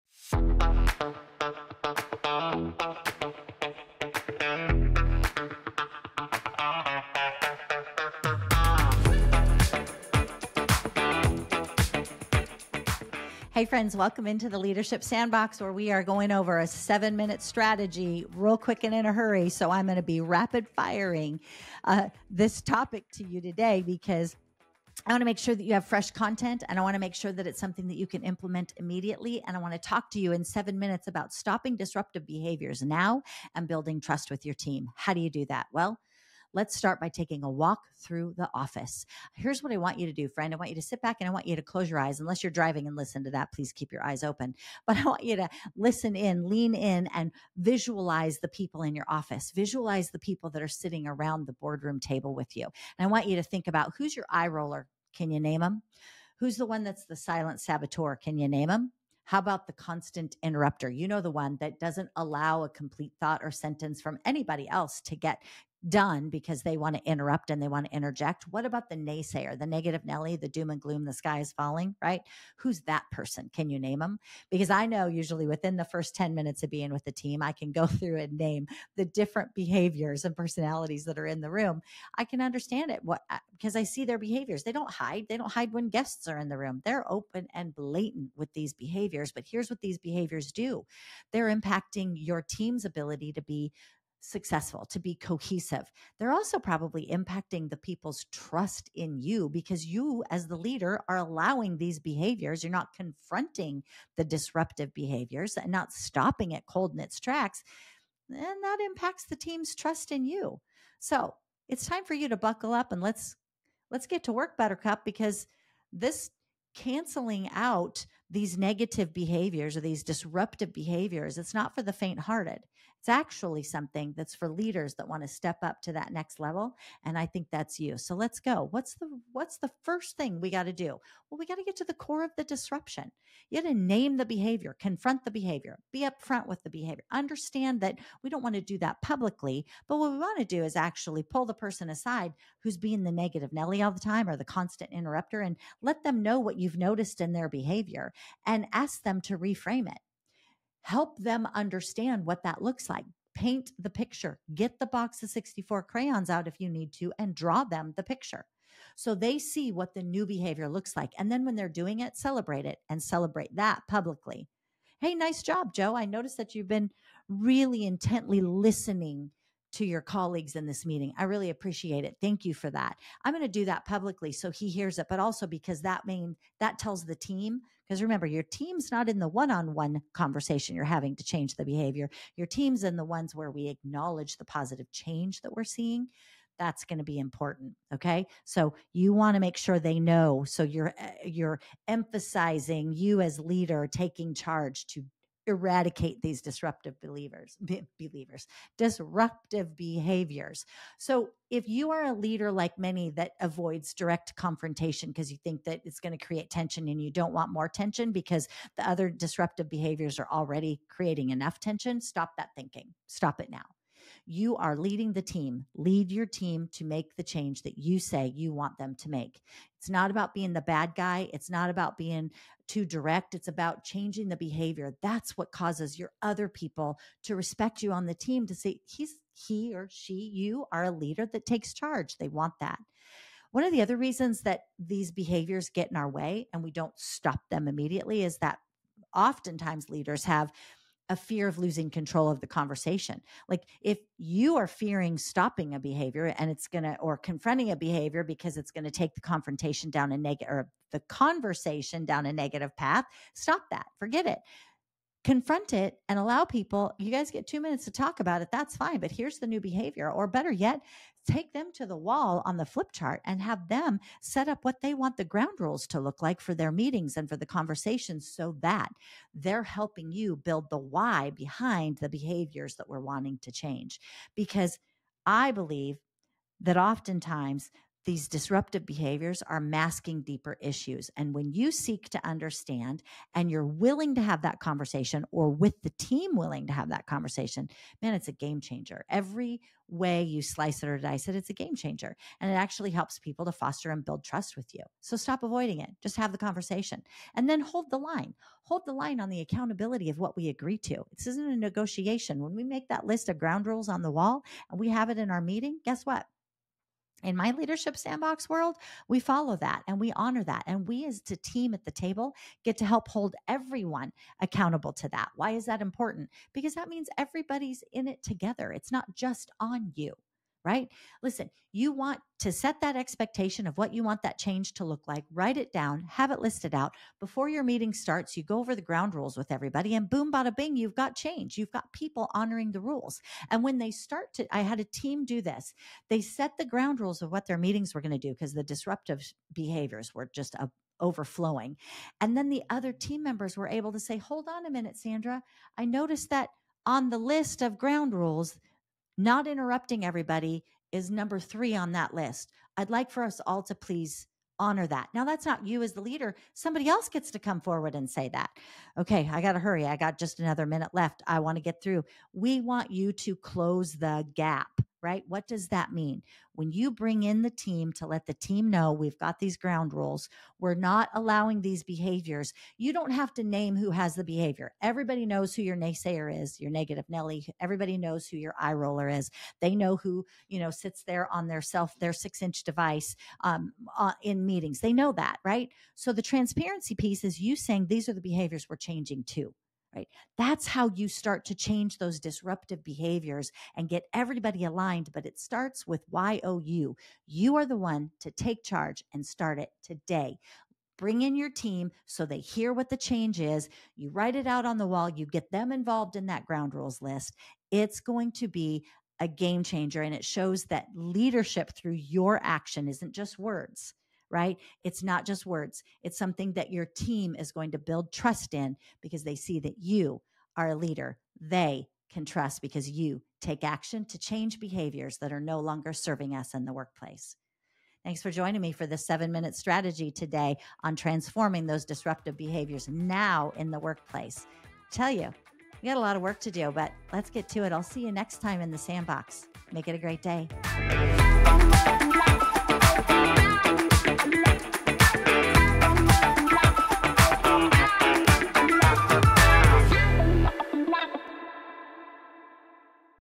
054: 7 Minutes to Stop Disruptive Behavior NOW and Build Trust
If you don’t take the time to address disruptive behavior, your team may begin to lose trust in you as a leader. How can you change this? In this short episode, I’ll share a heartfelt 7-minute strategy to help you overcome this leadership challenge.

1. The first step in stopping disruptive behavior is understanding its root cause. Pull your team member aside and ask them, with compassion, what’s really going on. Avoid addressing the issue in front of others, as this could cause embarrassment or resentment. A private, thoughtful conversation will show that you genuinely care about their well-being.

2. Don’t shy away from addressing issues head-on—it’s an essential part of leadership. Direct confrontation doesn’t have to create negative tension. When handled with care, it demonstrates your commitment to their growth and the team’s success. Confronting problems assertively, but kindly, shows that you are dedicated to fostering a positive, respectful environment. It earns the trust and respect of your team because they know you will guide them through challenges with sincerity.

3. Establishing ground rules for team interactions is vital for cultivating a sense of mutual respect. Write these rules down and review them before meetings to ensure everyone is on the same page. By involving the team in setting and upholding these standards, you create a sense of ownership, making it easier for everyone to hold each other accountable. This way, you share the responsibility, and it lightens the load for you as the leader.

4. Be open about your desire to create positive changes and involve your team in the process. When you follow through on your words with meaningful action, you demonstrate how much you value a respectful, collaborative environment. Leading by example is one of the most powerful ways to build trust and create a high-functioning team that feels truly cared for.

to change your mindset and behaviors so you can elevate your communication and leadership! This podcast is produced by TSE Studios.